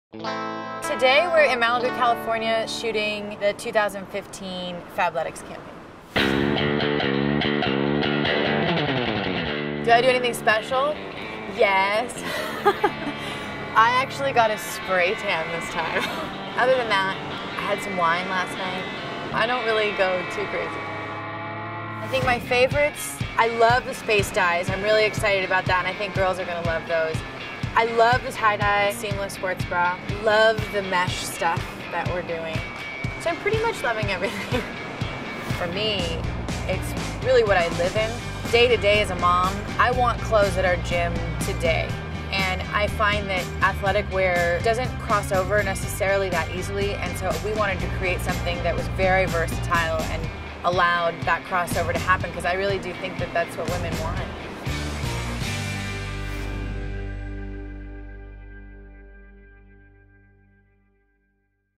Behind the Scenes with Kate Hudson
Behind the Scenes with Co-Founder Kate Hudson at the Fabletics Spring/Summer 2015 shoot.